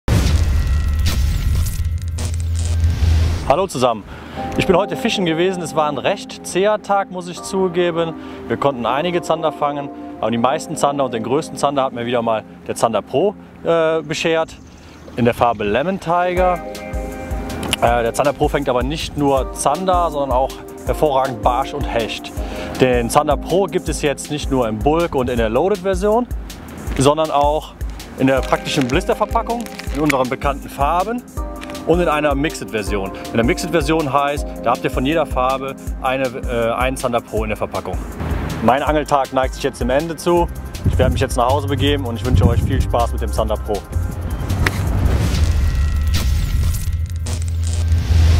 ***Fox Rage TV*** Zander Pro Blister Pack
Unsere Zander Pro Shad´s sind für ihre besondere Fängigkeit bekannt, selbst an schwierigen Tagen sind damit Fänge von Ausnahmefischen keine Seltenheit. Ab sofort ist der Zander ProShad auch in der praktischen Blisterverpackung erhältlich. In diesem Video gibt Pierre Johnen euch einen kleinen Einblick auf diese neue Verpackung.